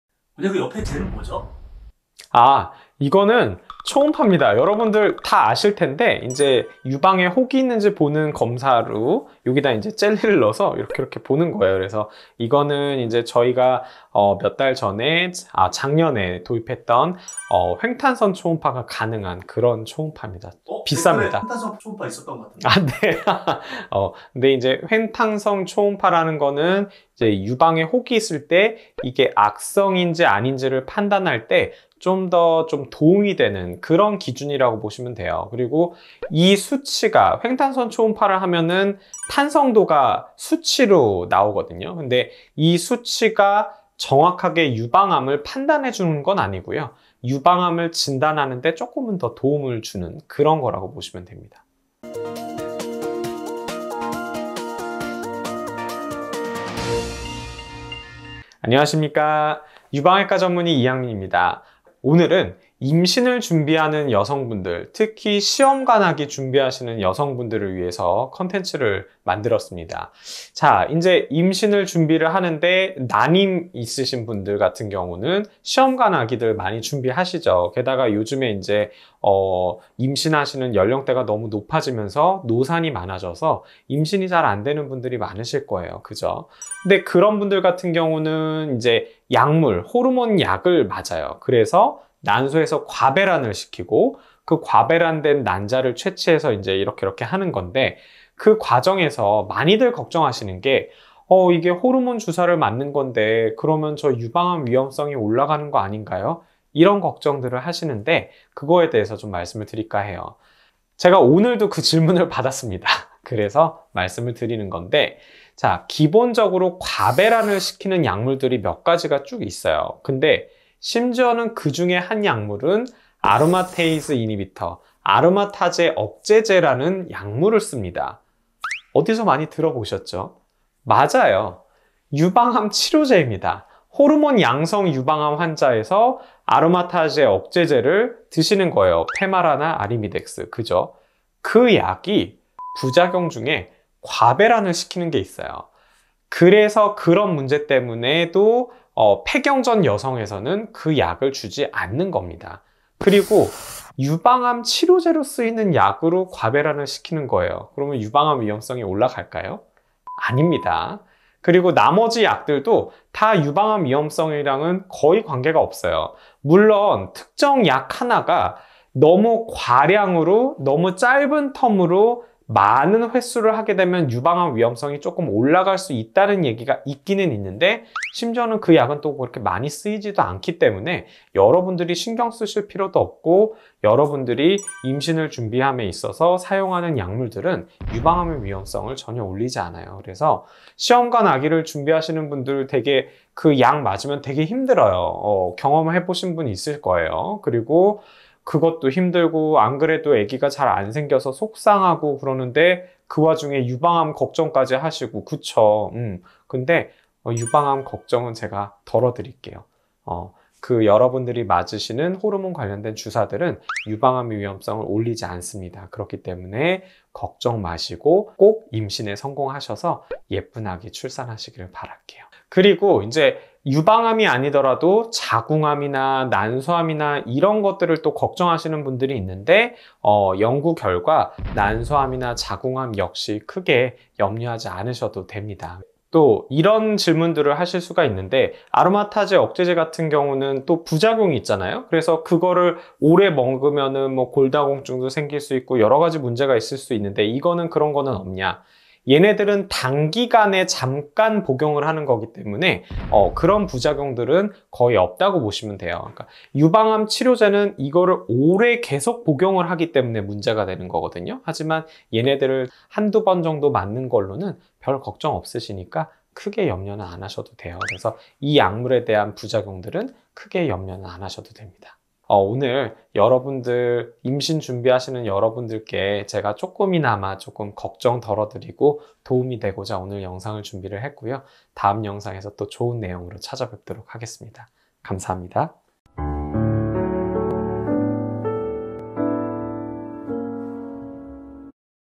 시험관 아기 준비중인데 유방암 발생을 올리나요
이쁜 아가를 원해서 시험관 시술을 하시는 모든 분들을 응원합니다.
시험관 시술을 결정하기까지도 많이 힘드셨을 텐데
시험관 시술을 할 때맞으시는 호르몬 약물들 때문에 많이들 힘드시죠.
조금만 더 참고 이겨내시면 원하시는 아가가 생길 거라 믿고 힘내세요!!!

지금 이 상황들도 힘든데 혹여나 호르몬 주사를 맞으면 유방암 위험성이 올라가는 거 아닌가 하고
걱정하시는 분들이 많으세요!!!

아닙니다!!! 걱겅하지 마시라고 "임신을 위한 시험관 시술과 유방암"에 관해 영상을 준비해 보았습니다.

영상과 댓글을 통해 여러분의 여성건강을 지켜드리고자 최선을 다하고 있는
더연세유외과 대표원장 이 학 민 입니다.